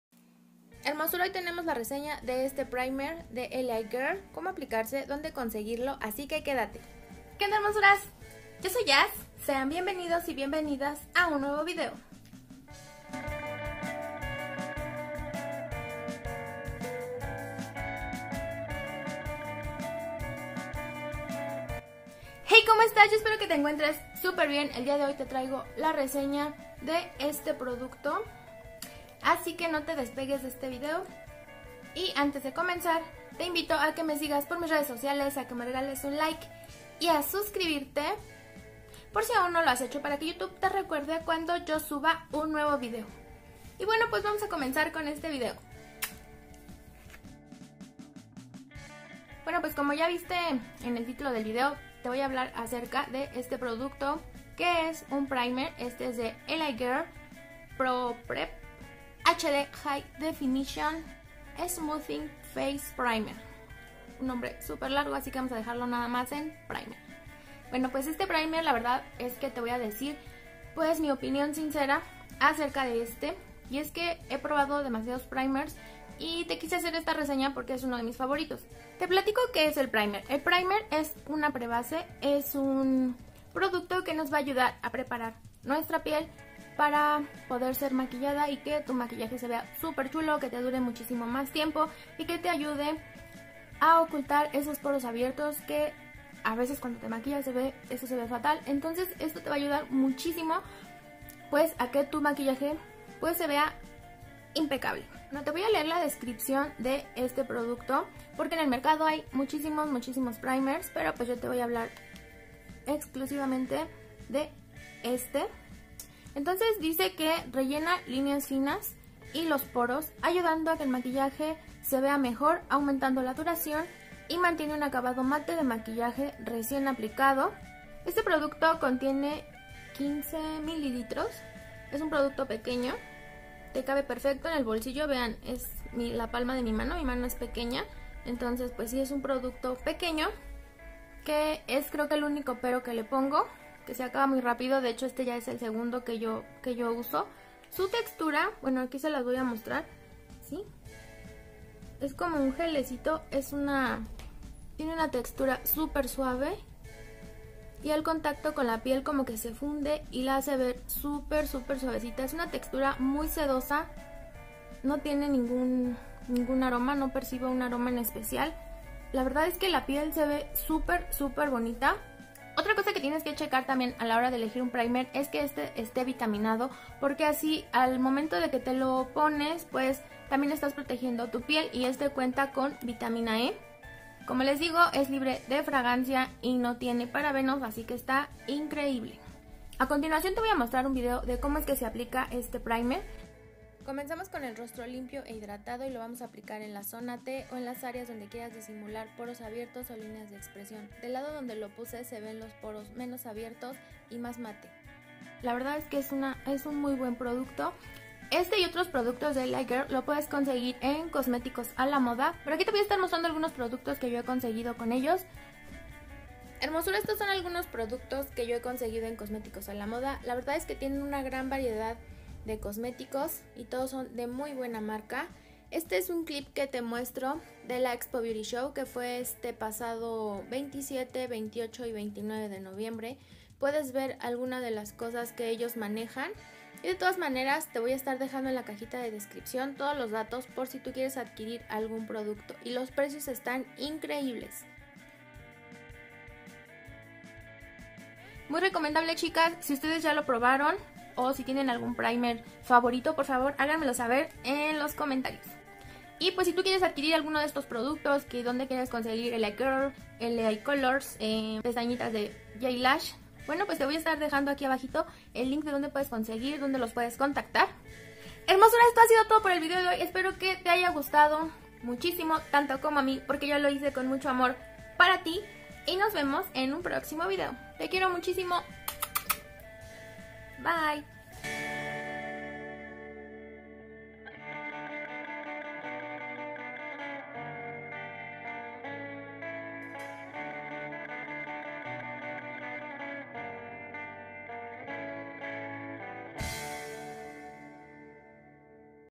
LA Girl Pro Prep Primer Reseña / el mejor primer economico / Cosmeticos a la moda
🌟 HOLA HERMOSURAS! 💗

Espero te encuentres muy pero muy bien!

El día de hoy te traigo la reseña de un Primer, que esta genial, me encanta y es el LA GIRL PRO PREP, este lo puedes encontrar en Cosmeticos a lo Moda, te dejo los links donde puedes conseguir tu primer: